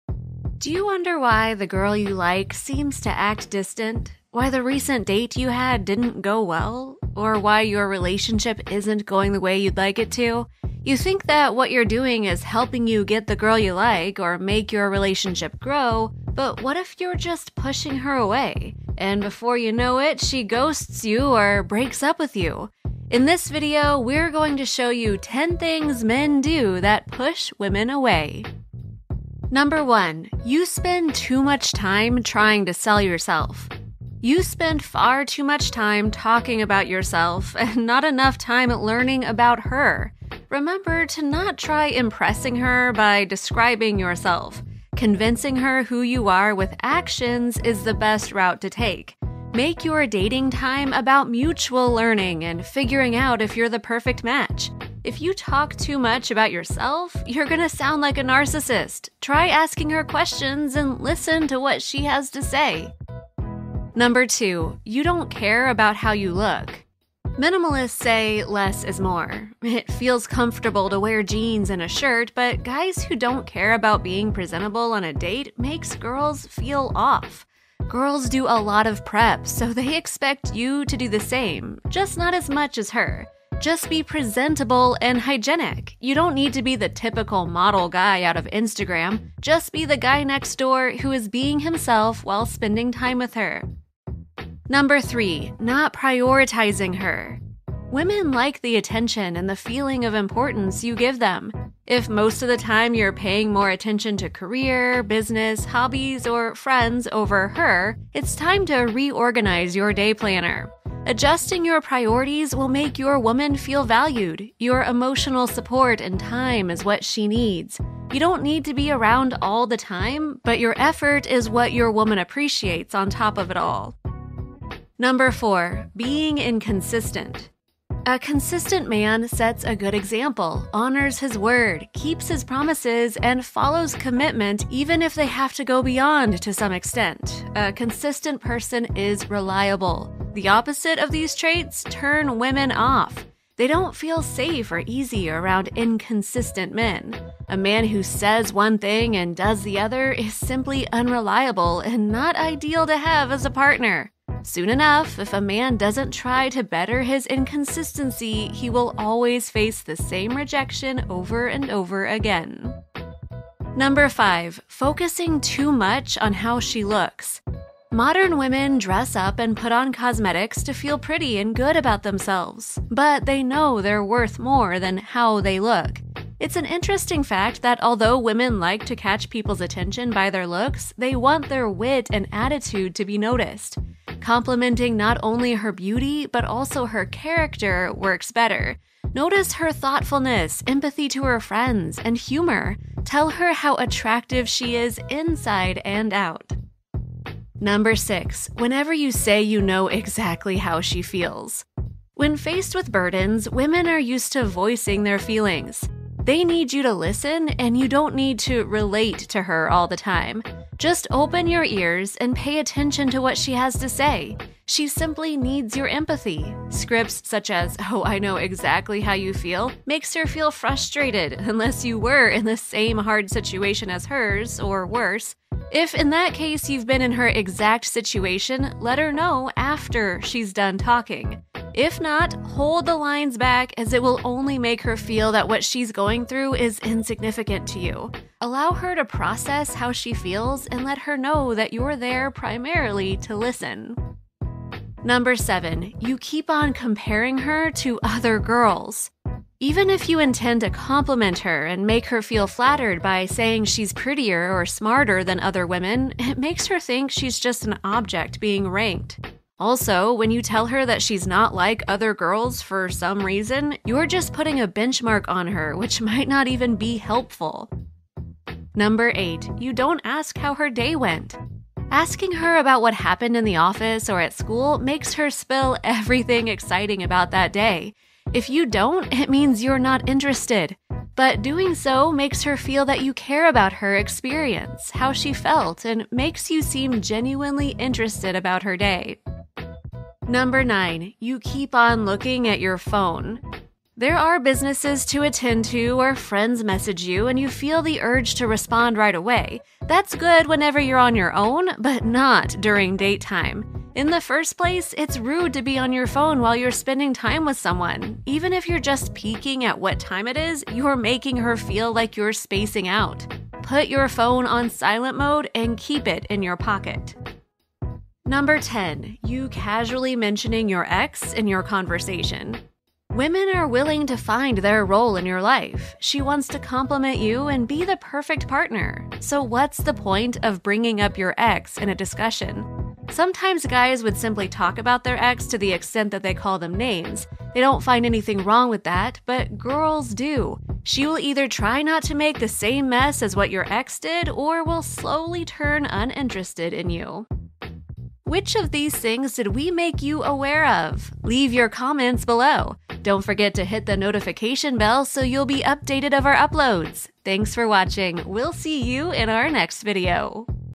10 THINGS Men do that PUSH WOMEN AWAY
Do you wonder why the girl you like seems to act distant? Why the recent date you had didn't go well. Or why your relationship isn't going the way you want it to. You think what you're doing is helping you get the girl you like or make your relationship grow, but what if you're just pushing her away? And before you know it she ghosts you or breaks up with you. In this video, we're going to show you 10 things men do that push women away.

Chief Editor: Paul Mendez
Script Writer: Eka Watson
Voice Over: Teagan Mckenzie